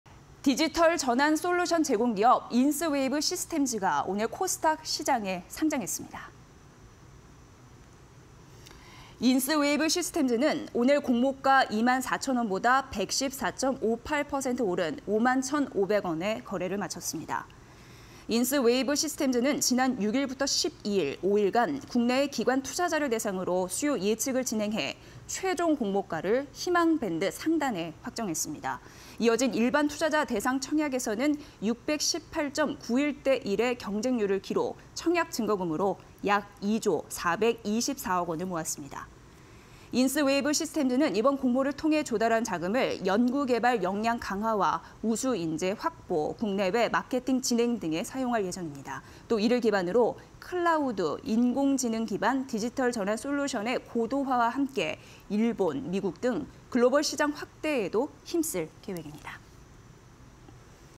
인스웨이브시스템즈, 코스닥 상장… "R&D 강화·글로벌 진출 박차" (20230925)
인스웨이브시스템즈, 코스닥 상장…"R&D 강화·글로벌 진출 박차"